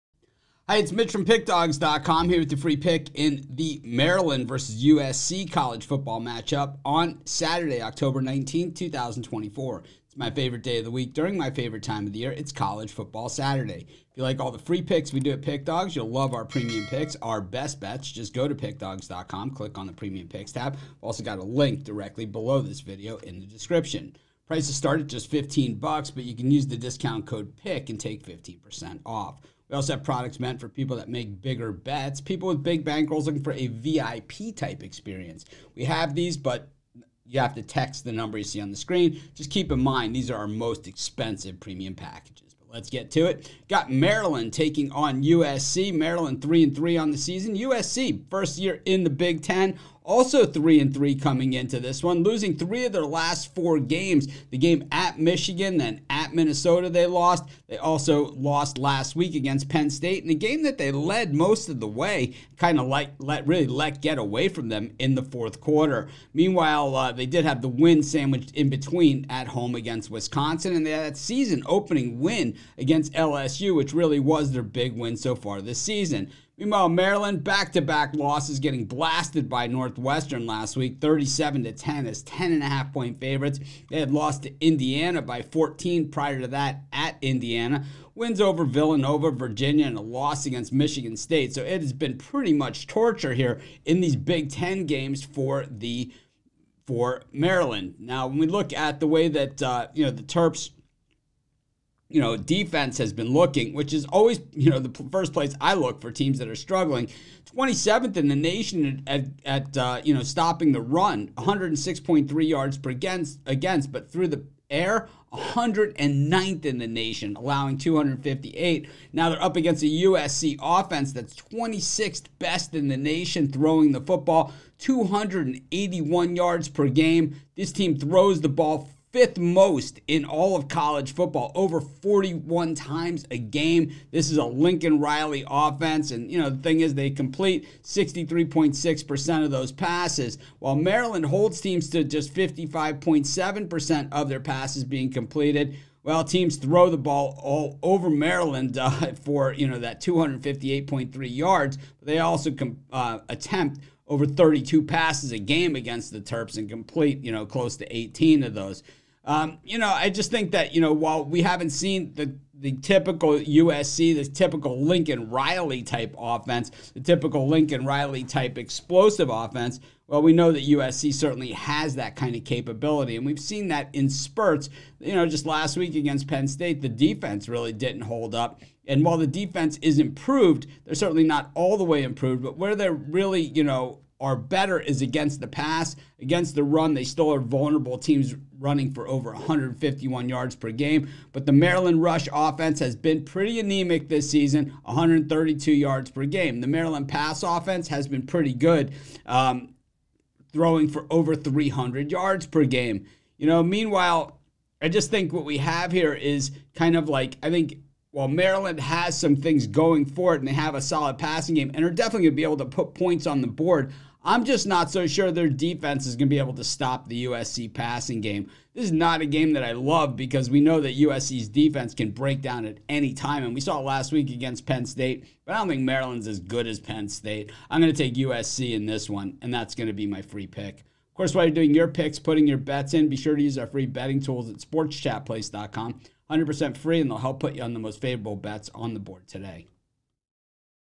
Maryland vs USC 10/19/24 College Football Picks & Predictions | Week 8 NCAAF Betting Tips
The USC Trojans take on the Maryland Terrapins in Week 8 of College Football Action. Get Mitch's College Football Pick and Prediction for this game.